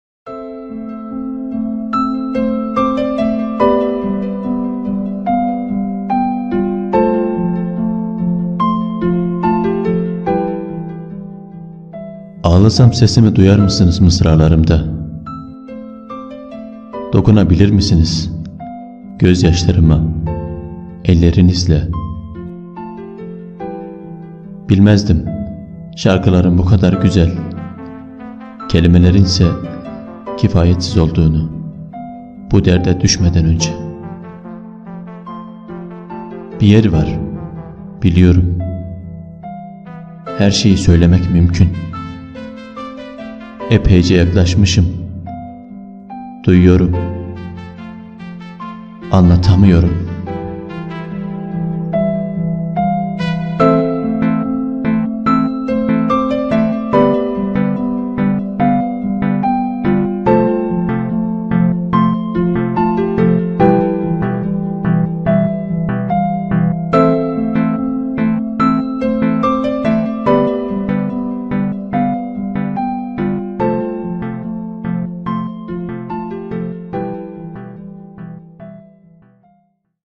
Orhan Veli Kanık | Anlatamıyorum
Ağlasam sesimi duyar mısınız, Mısralarımda; Dokunabilir misiniz, Gözyaşlarıma, ellerinizle? Bilmezdim şarkıların bu kadar güzel, Kelimelerinse kifayetsiz olduğunu Bu derde düşmeden önce. Bir yer var, biliyorum; Her şeyi söylemek mümkün; Epeyce yaklaşmışım, duyuyorum; Anlatamıyorum...